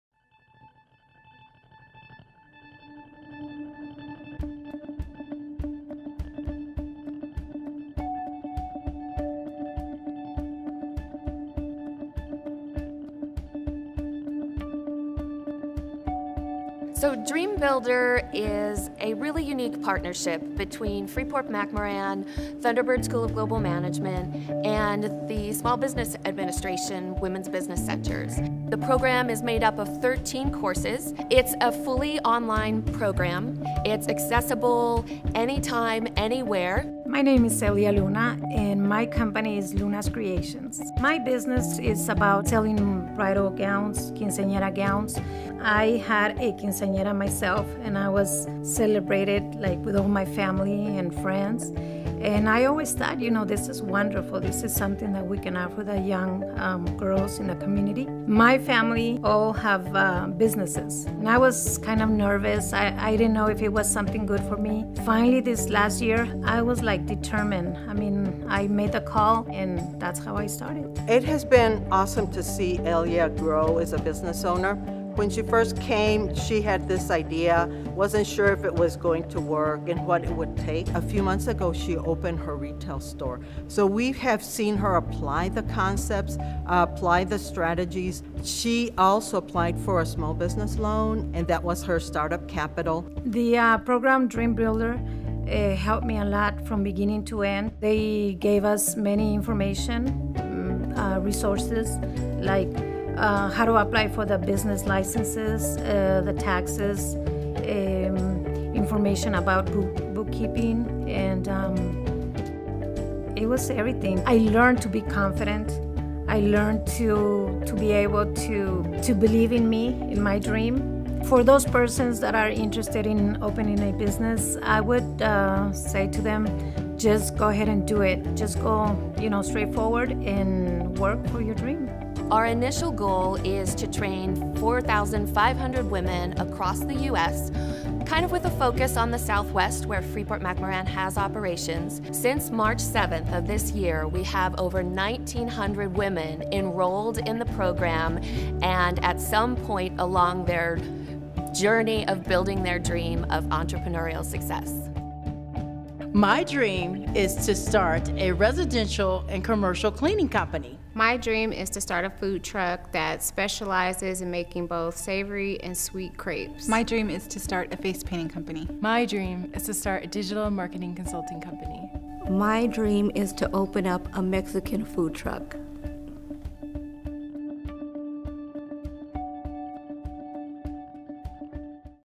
Dream Builder: The Women's Business Creator - CGI America 2015 Progress Report
A 2011 CGIAmerica commitment by Freeport-McMoRan Inc. is providing business skills training through its Women's Business Academy to women planning to start or grow small businesses. Learn more about this CGI Commitment to Action